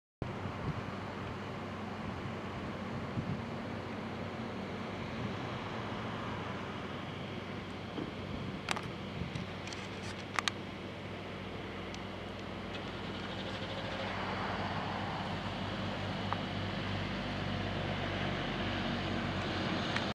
fallbrook structure fire 8-29-2014
Structure fire involving house in Fallbrook California. estimate loss from damage approx $90k